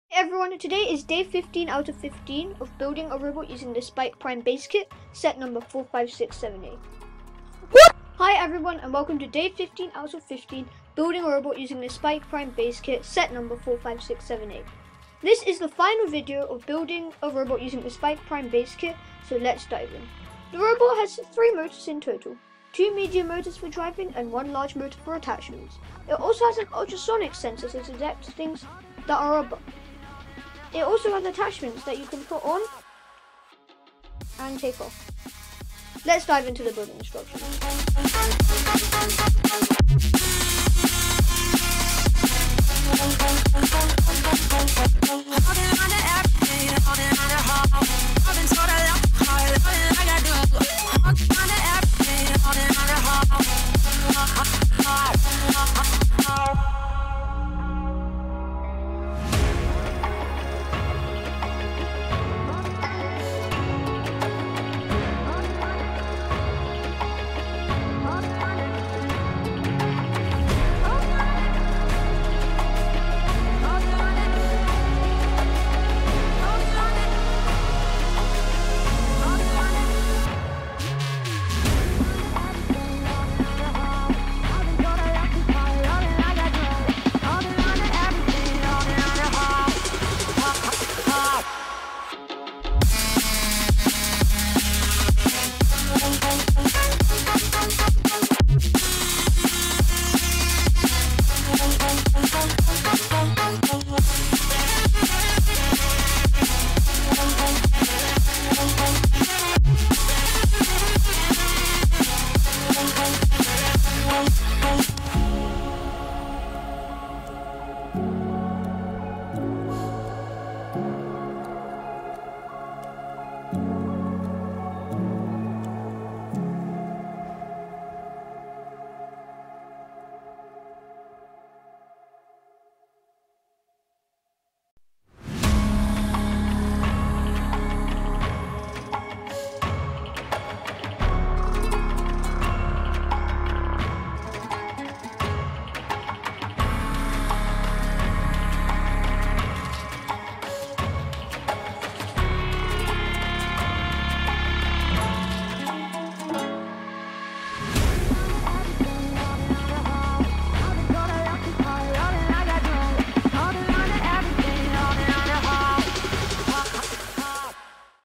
Day Fifteen - Robot and Attachment Design for FLL Submerged
Today is Day 15 out of 15 of building a robot using the SPIKE Prime base kit, set number 45678. This is the final video using the SPIKE Prime base kit. Tommorow we will start using the expansion kit.